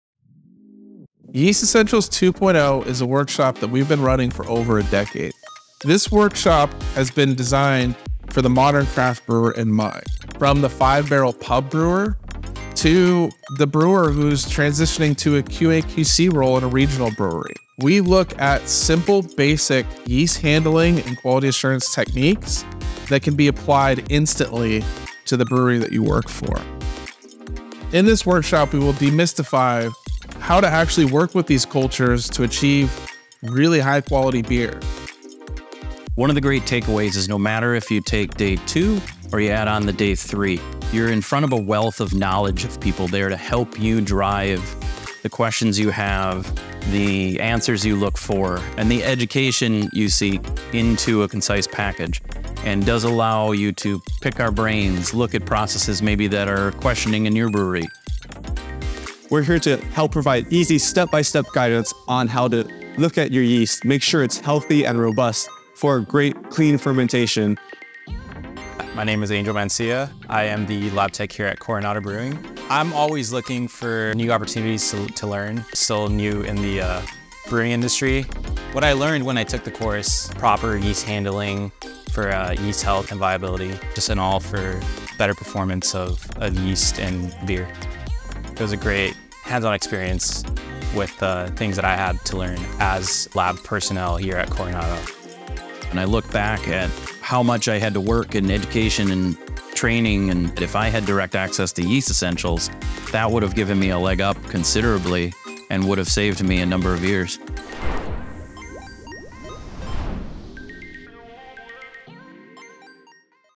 Yeast Essentials - Master Your Fermentation: One High-Impact Class, Two Incredible Locations
Master Your Fermentation: One High-Impact Class, Two Incredible Locations
Step beyond the basics and dive deep into the science of fermentation in our practical, hands-on 2-day (or 3-day!) Yeast Essentials Workshop, designed for all beverage-makers who are serious about upping their game. Whether you're refining your yeast harvesting practices or starting a quality control program, this workshop delivers powerful tools and advanced techniques to elevate your fermentation operations.

Immerse yourself in a vibrant craft beer scene by the Pacific, surrounded by cutting-edge breweries and sunny coastal vibes.

Asheville, NC
oin us in the heart of the Blue Ridge Mountains, where innovation meets tradition in one of the country’s most creative brewing hubs.

What You’ll Learn:
-Yeast handling best practices from industry experts
-Troubleshooting fermentation issues with confidence
-Yeast cell counting, viability testing, and microbiological plating
-Real-world QA/QC methodologies and guidelines
-Technical brewhouse demos you can’t get anywhere else

Course Lectures & Labs Include:
-Introduction to Yeast and Fermentation
-Strain Selection & Beer Styles
-Keys to a Healthy Fermentation
-Setting Up a Brewery Lab
-Alternative Beverages & New Market Trends
-Yeast Pitching, Repitching & Optimization
Live Hands-on Demos: Cell Counting, Micro Plating, and more

Bring your team and save 10% on 2+ seats or 20% on 5+—no code needed.
Limited spots available—let’s unlock the full potential of your yeast handling!